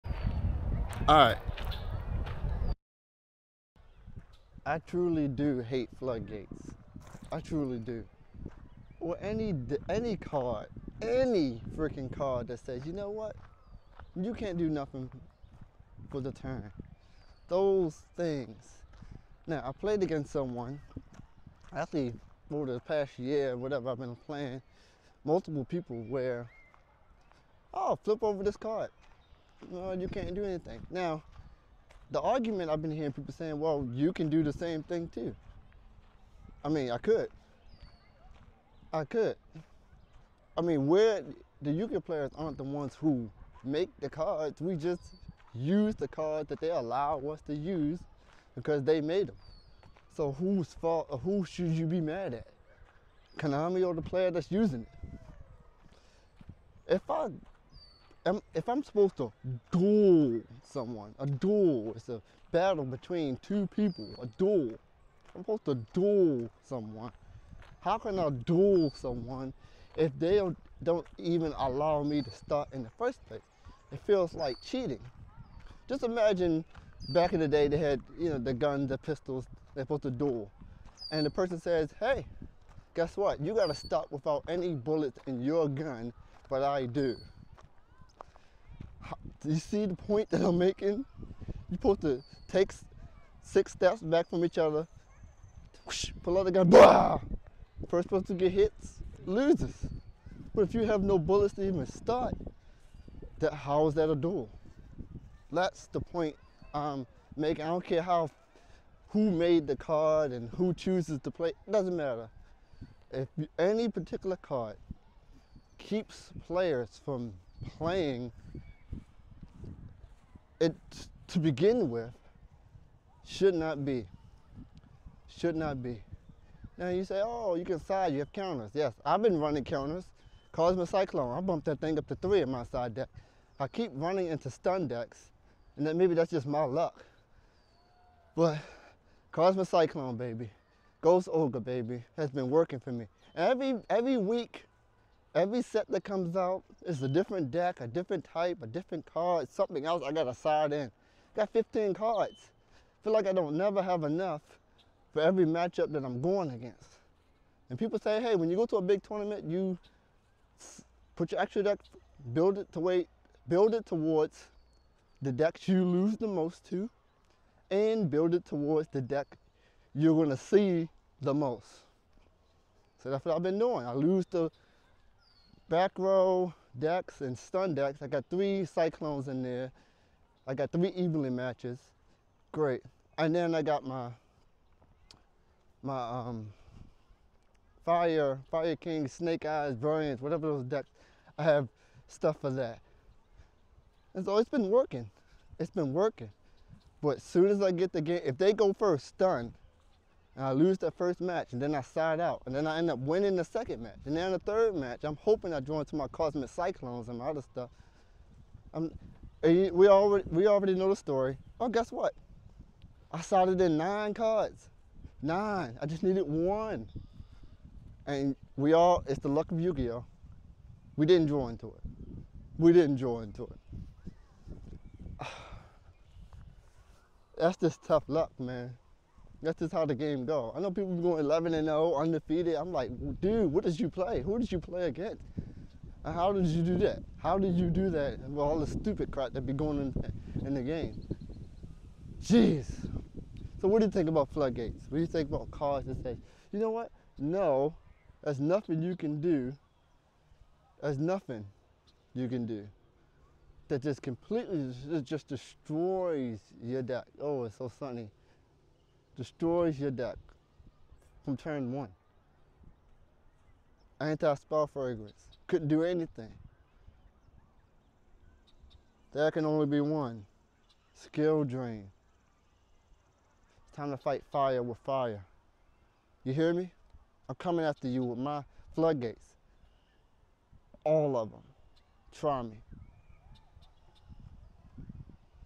I Hate Yugioh Floodgates!
Floodgates can be the worst sometimes lol. Yeah, I had a bad week with them.